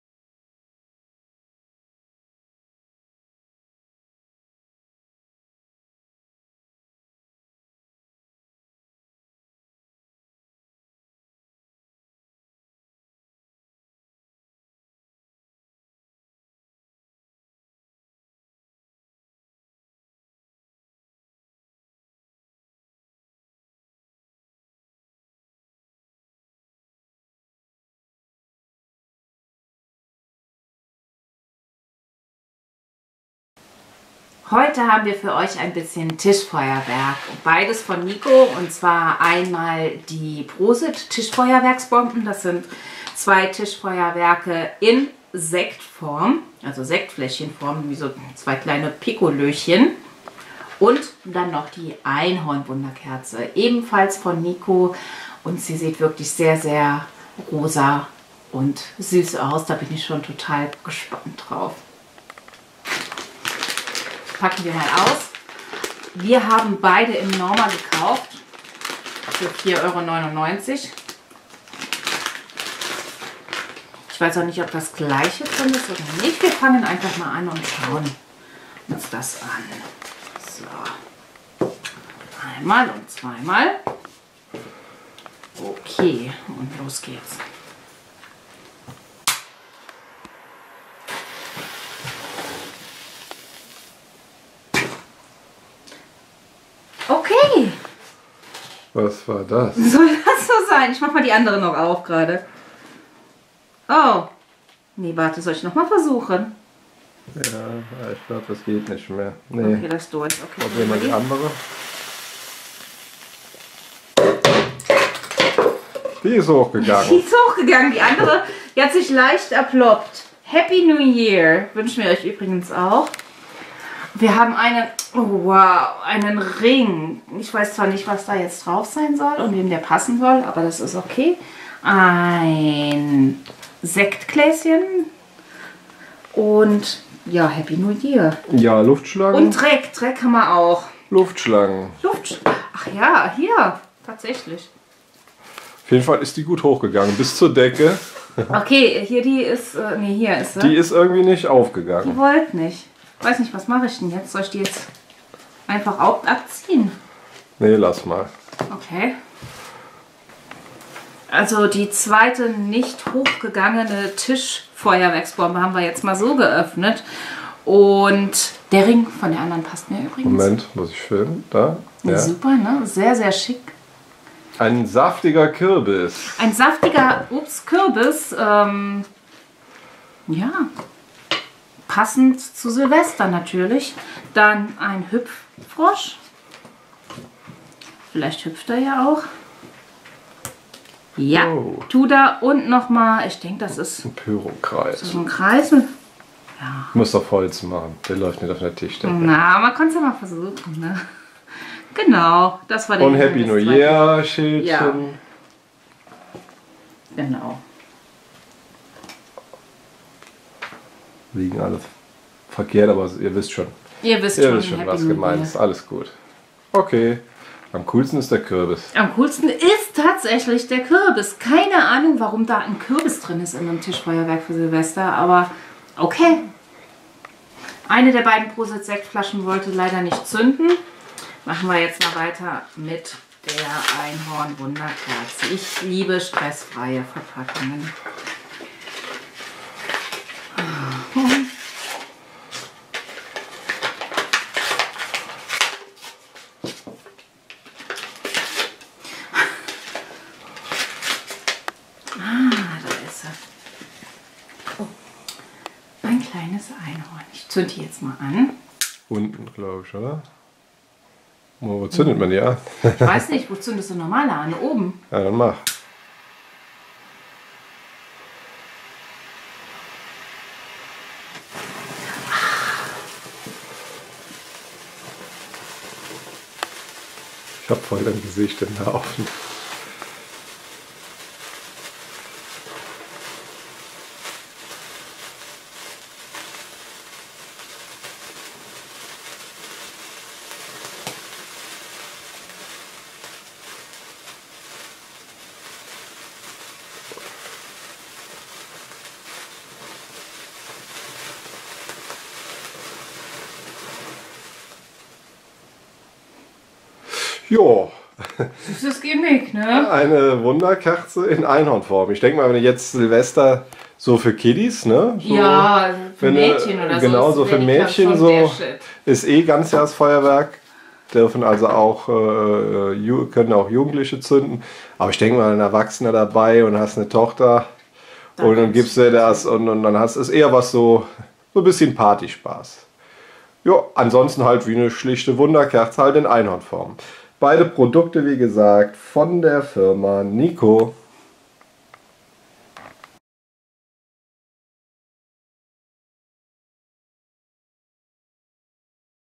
Nico Einhorn-Wunderkerze & Prosit Tischbomben
Wir testen ja bekanntlich auch Tischfeuerwerk für die Silvesterparty. Heute haben wir uns die Einhorn-Wunderkerze und die Sektflaschen-Tischbomben von Nico vorgeknüpft.